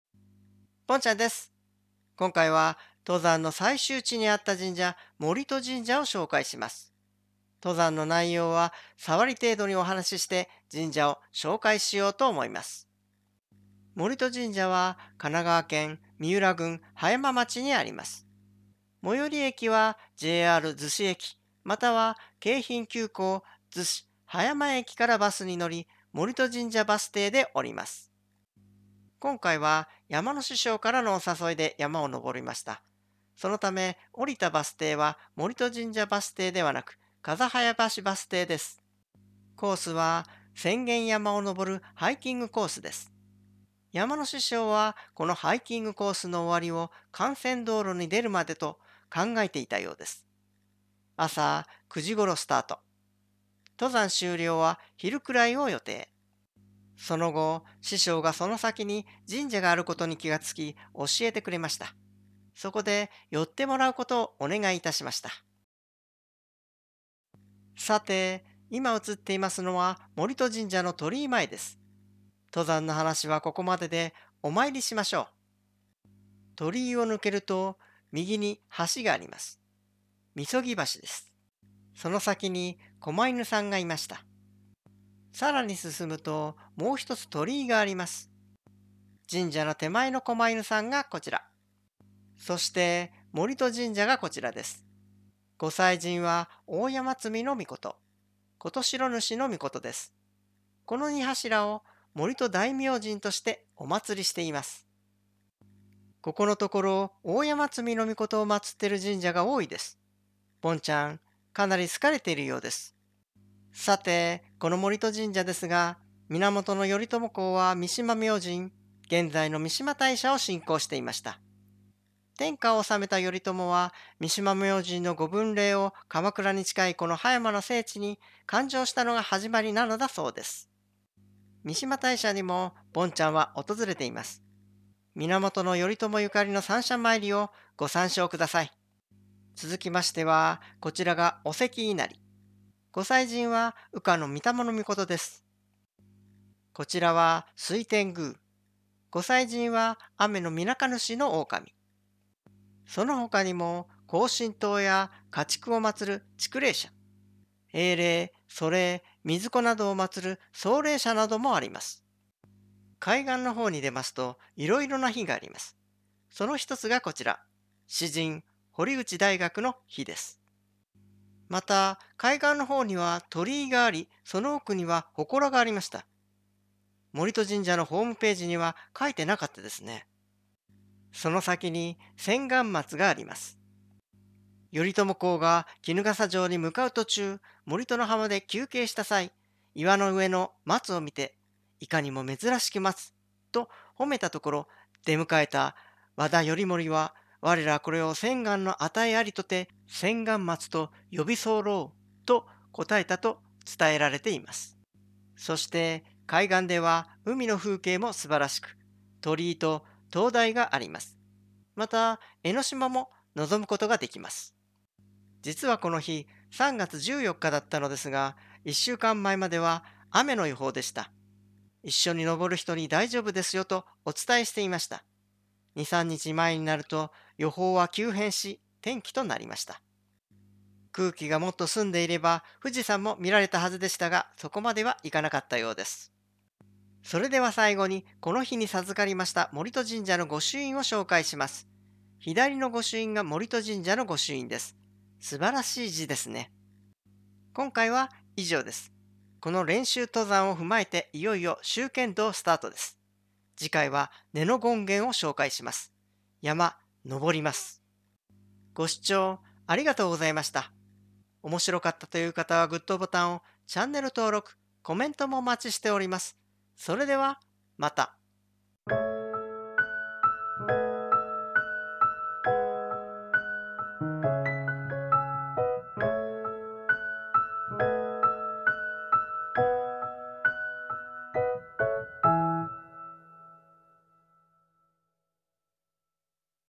e38 葉山にある 源頼朝ゆかりの神社 森戸神社
今回は、山の師匠からのお誘いで行った登山の最終地に
あった源頼朝ゆかりの神社　森戸神社を紹介します。

登山は、高いところで標高約１９０ｍと低い山。
丁度、これから修験道のシリーズも再開する予定だったので、
良い練習登山と思い参加。
山の師匠も神社までは考えていなかったようで、
ルートの最後の辺りに神社があるのを
後に師匠が見つけ「神社あるよ。」と言われ
寄ってもらいました。

登山の話は、さわりだけで
森戸神社を重点にこちらもさらっと紹介します。
そして、そこの神社の御祭神は、
またしても、あの神様でした。

それでは、お楽しみください。

源頼朝ゆかりの三社参りの動画はこちら

静御前さんのお墓の動画はこちら

源義経ゆかりの神社　白旗神社の動画はこちら

弁慶塚があるお寺の動画はこちら

森戸神社のHPはこちら